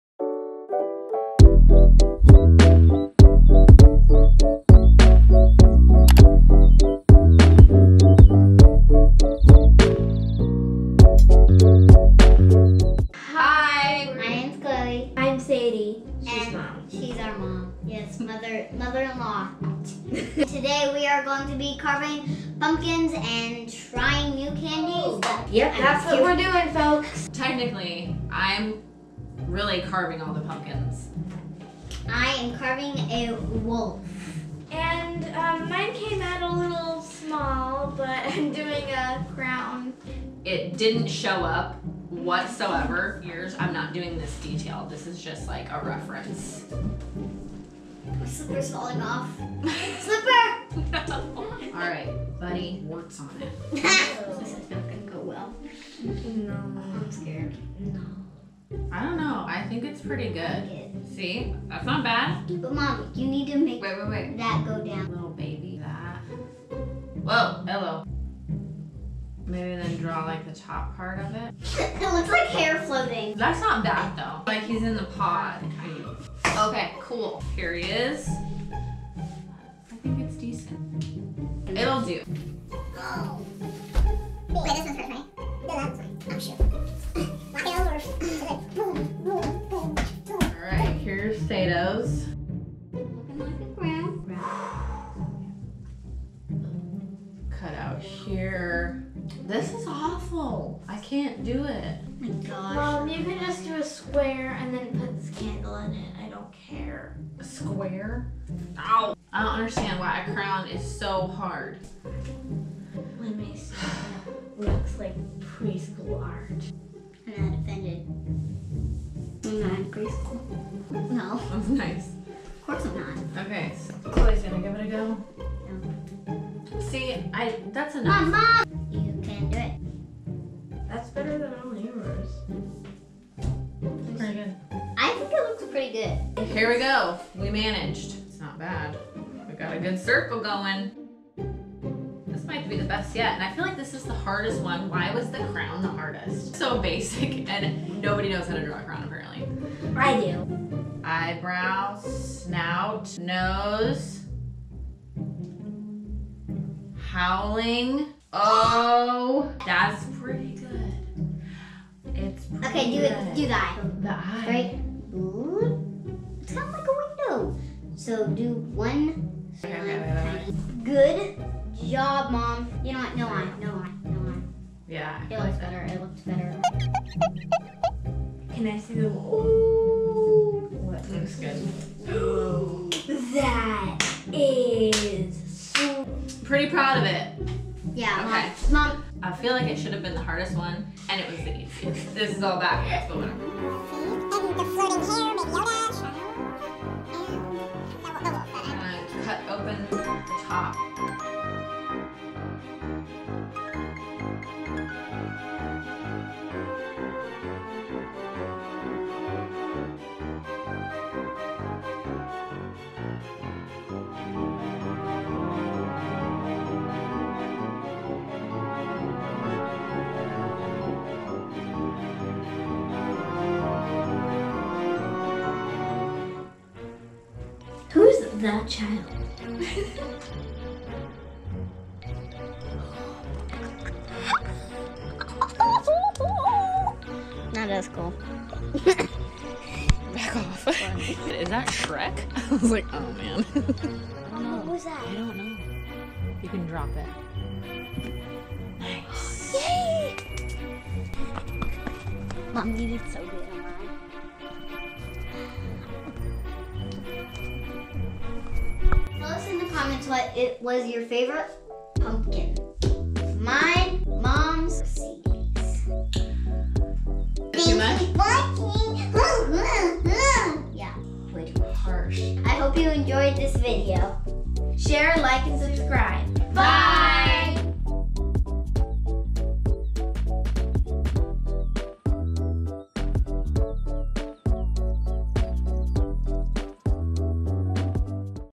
Fall | Halloween part 1 | Pumpkin carving | 2020 | Star Wars The Child, Wolf, & Crown
Every year we HAVE to carve some pumpkins!!
This year was probably my best and also my worst pumpkin carvings yet! Thanksssss!

My girls did not go easy on me while carving theses...